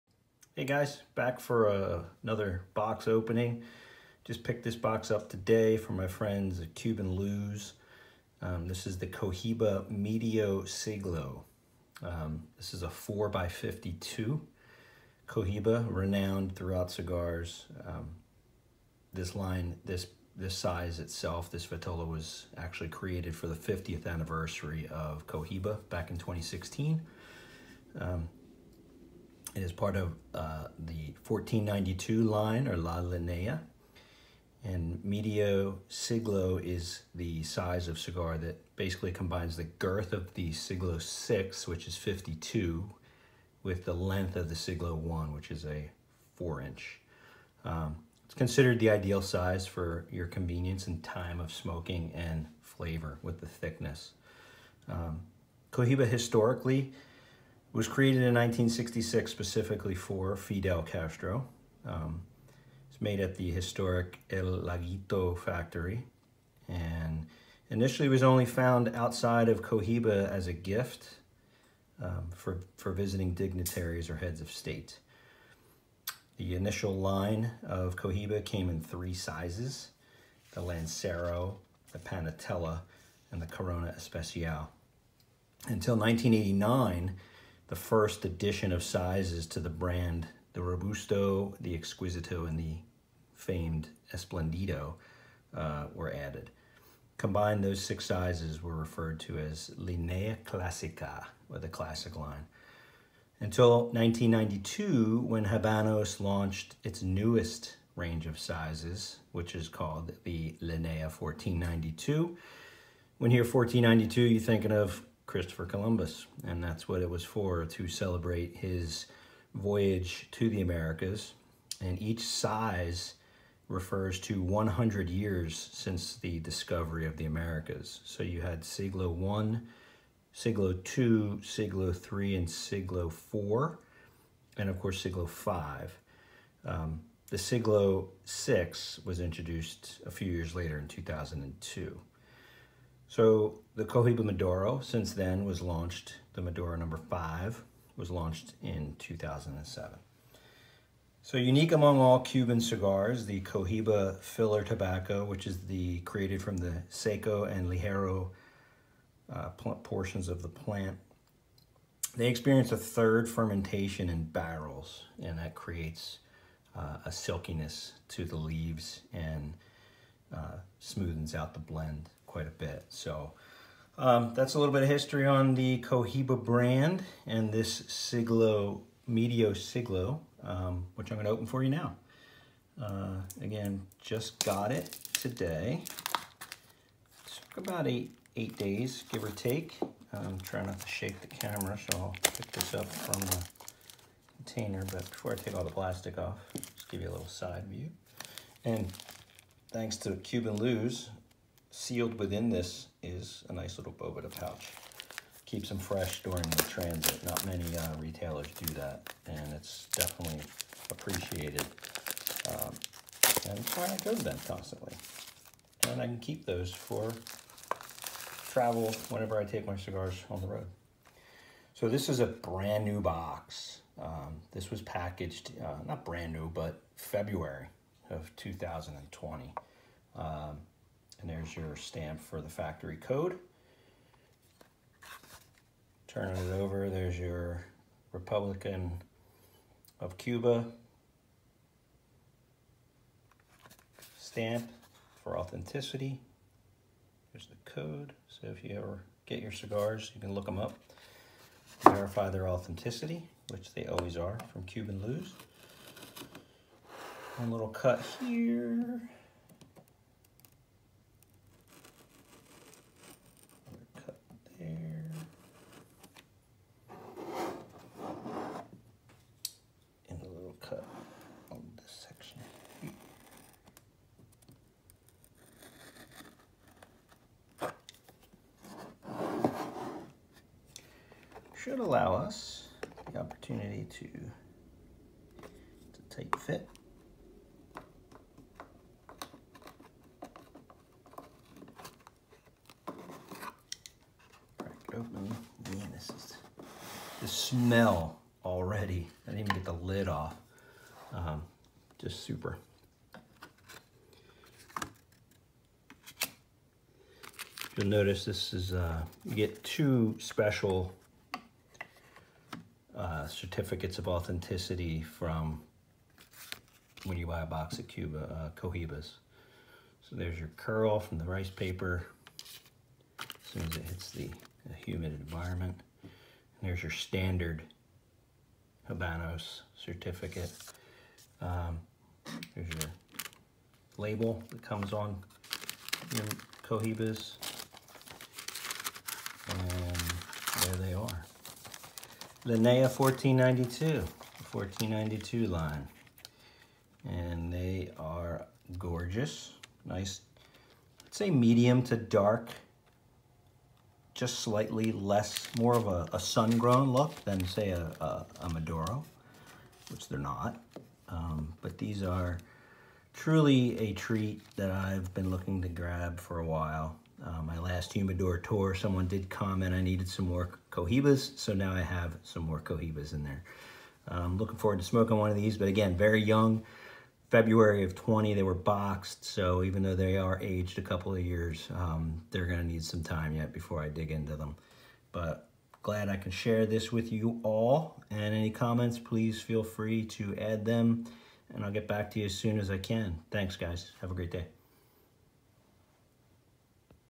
Cohiba Medio Siglo Cuban Cigar Box Opening
The Cohiba Medio Siglo is a 4 x 52 from the La Linea 1492 which debuted in 2016 to celebrate the 50th Anniversary of Cohiba and was the first addition to the Linea 1492 since the Siglo VI was released in 2002.   The Linea 1492 originated in 1992 to celebrate Christopher Columbus discovering the Americas. Each cigar produced from the line represented 100 years since the discovery starting with the Siglo I, II, III, IV and V with the Siglo VI arriving in 2002. The Medio Siglo combines the girth (52) of the Siglo VI with the length (4) of the Siglo I resulting in a well balanced and flavorful smoke.